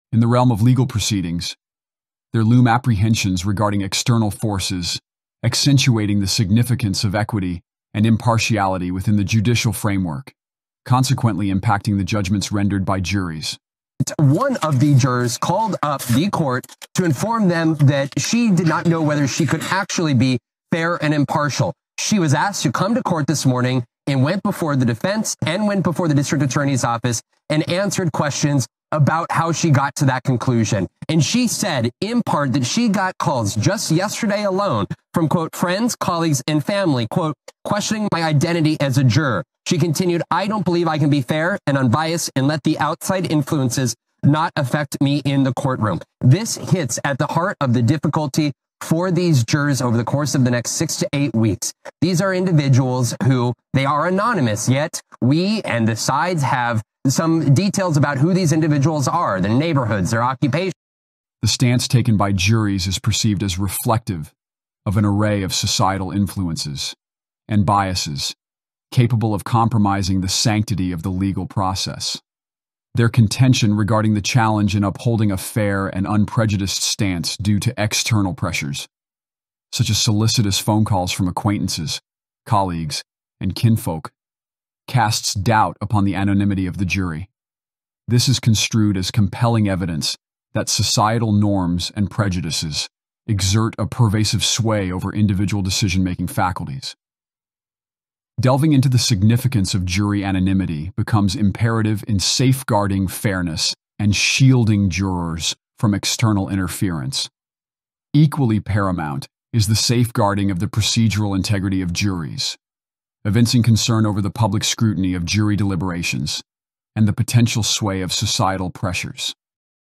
Donald Trump, Juror's Admission Revealed
In this video, we delve into the recent admission by a Trump juror regarding their inability to remain fair and impartial. Learn about the challenges faced by jurors amidst external influences and how it impacts high-profile trials. Stay informed with our in-depth analysis.